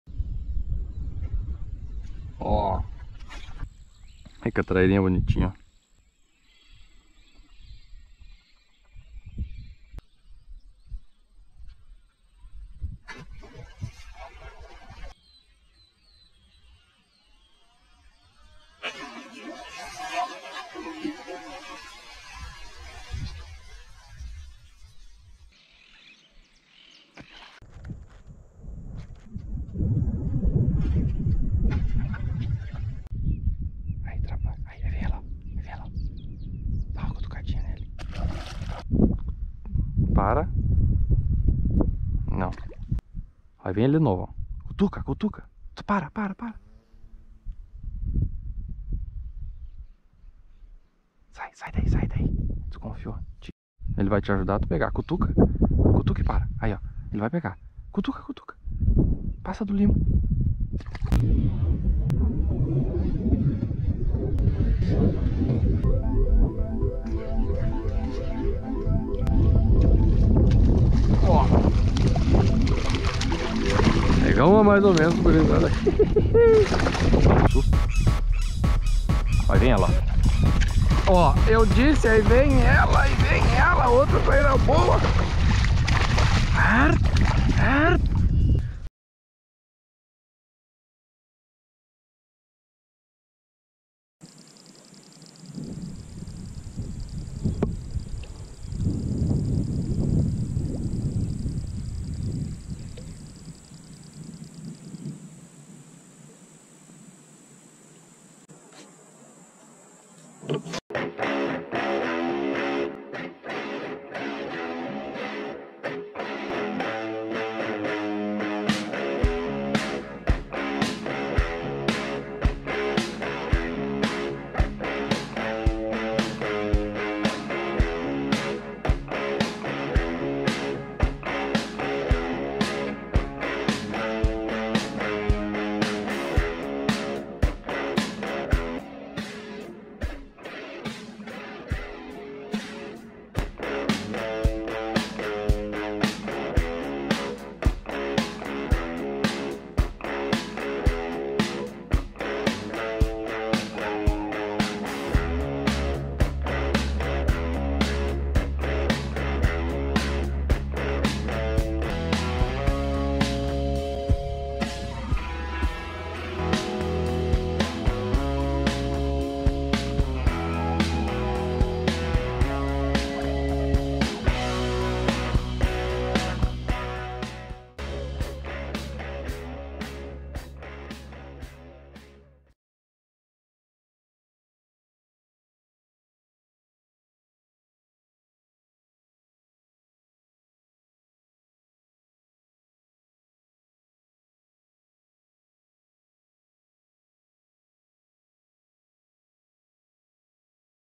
INCRIVEIS ATAQUES DE TRAÍRA NA SUPERFICIE pescaria de frog sapo
Um video de pesca de compilado de ataques de traira na superficie. Só incriveis ataques de traíra nas iscas sapo, frog.

Pescaria, pesca, traira, trairão, dicas de pesca, carretilha, molinete, video de pesca